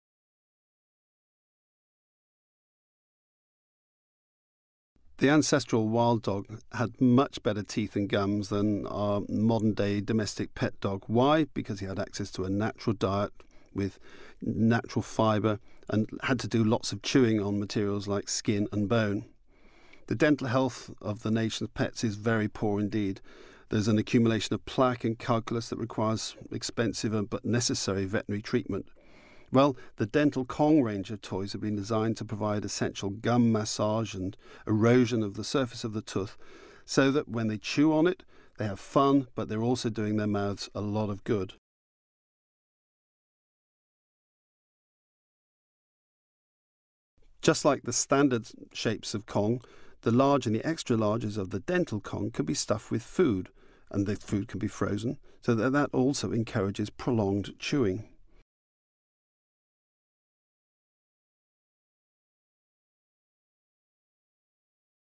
Kong Dental Juguete Perro
Gracias al Kong Dental ayudaremos a nuestro perro a mantener unos dientes limpios y sanos. Realizado con goma Kong resistente para poder soportar las sesiones de juego y limpieza.